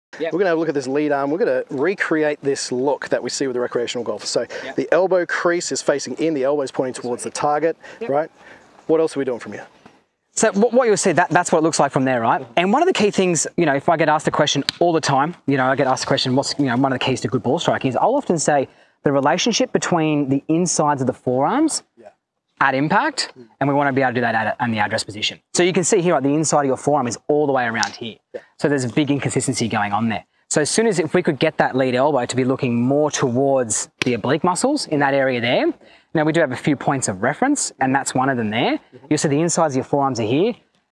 Relationship Between the Forearms
In this video, Kerrod and Toby will show you the importance of relationship between the forearm and simple yet effective way to cure one of the worst flaws in golf.